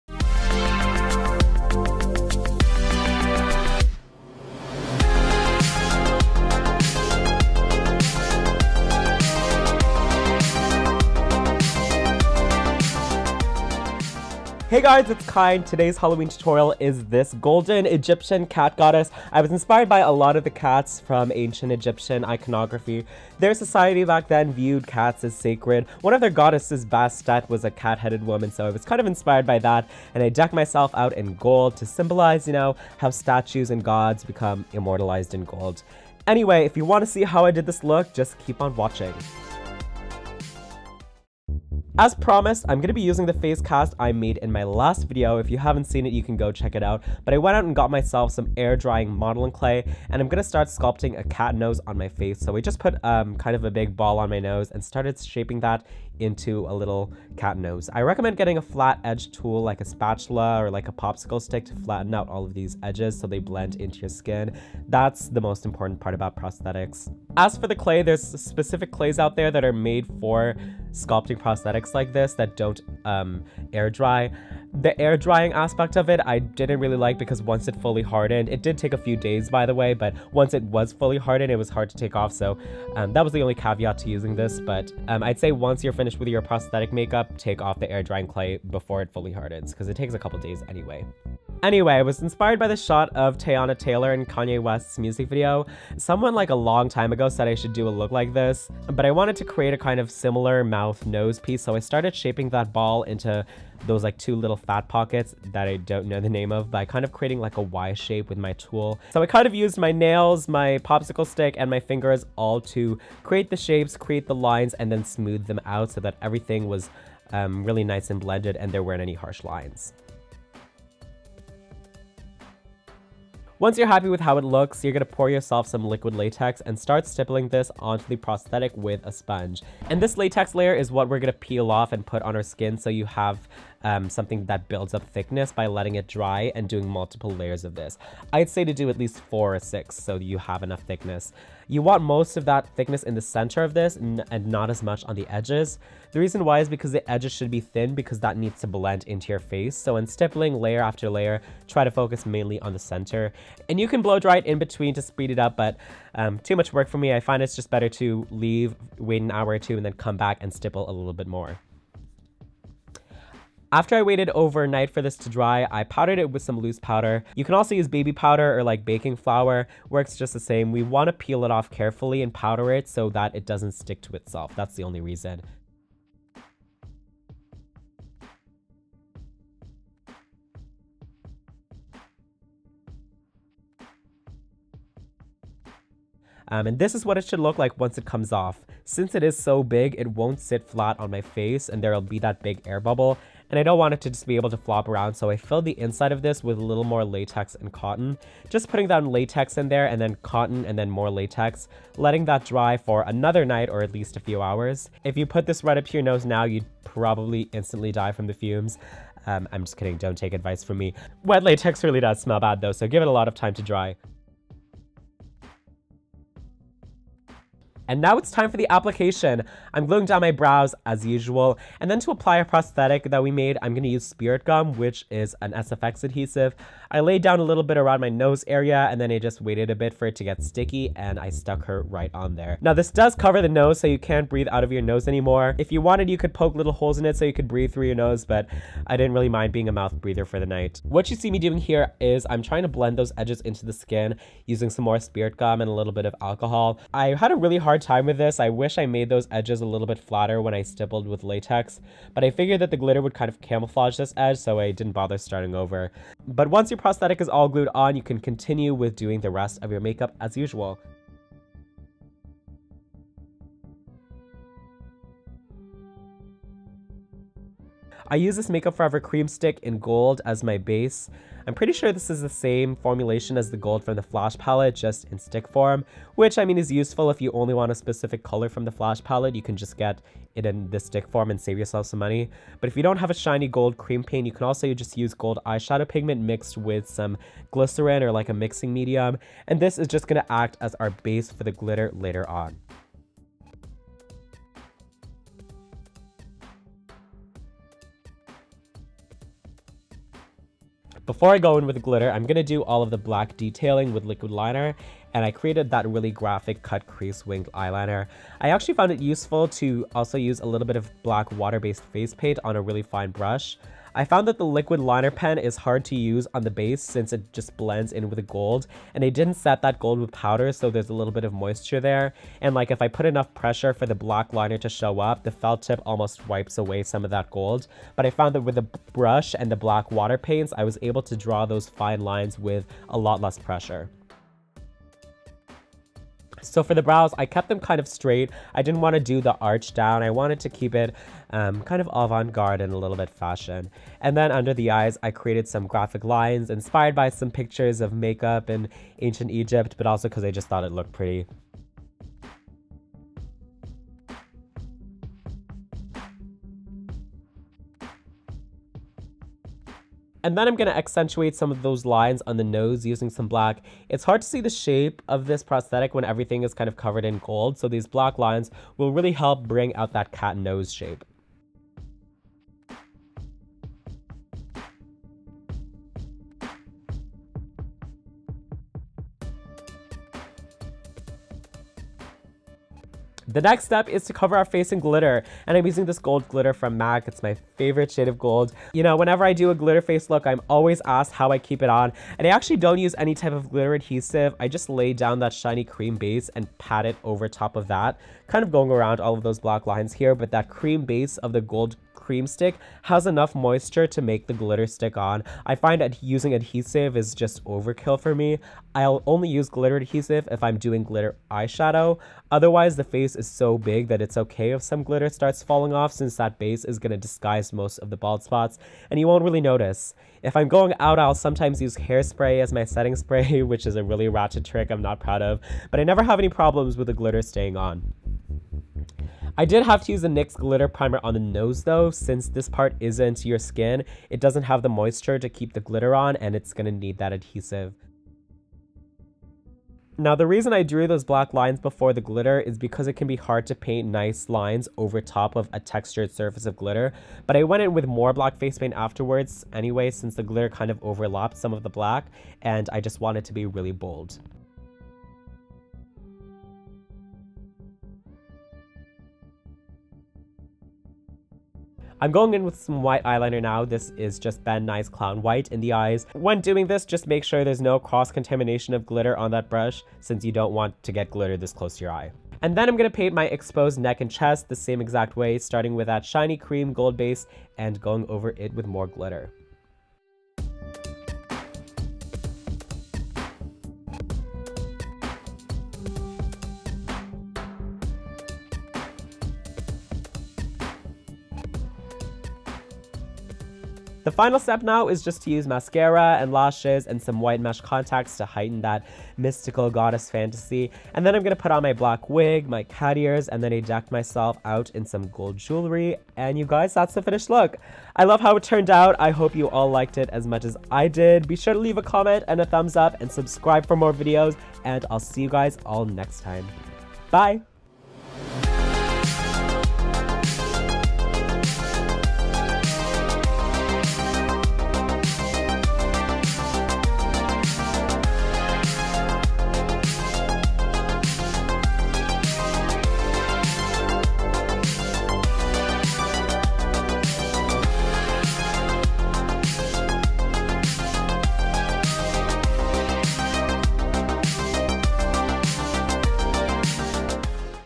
BASTET CAT GODDESS MAKEUP TUTORIAL | ONLINEKYNE HALLOWEEN 2017
Today's makeup tutorial is this golden glitter cat inspired by the Ancient Egyptian goddess of cats Bastet!

Products used:
- Face cast
- Liquid Latex
- Modelling clay
- Cotton balls
- Spirit Gum
- Makeup Forever Cream Paint Stick - Gold
- Black Liquid Eyeliner
- Wolfe FX Black Hydrocolour
- MAC Gold Glitter
- Camoeyes White Mesh Lenses